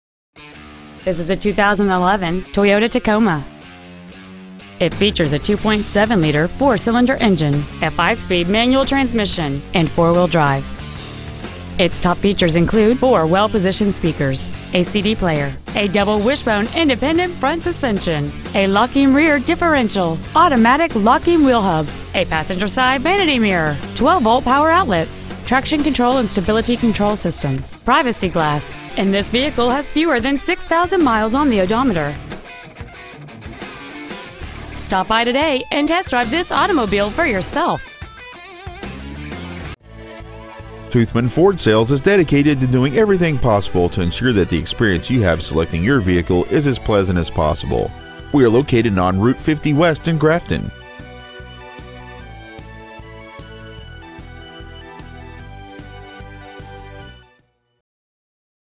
2011 TOYOTA TACOMA WV
We've been honored to serve the  WV area , we promise that your experience at our dealership will exceed your expectations !

 Year : 2011
 Make : TOYOTA
 Model : TACOMA
 Engine : 2.7L I4
 Trans . : MANUAL 5SPD
 Exterior : GREEN
 Miles : 5,812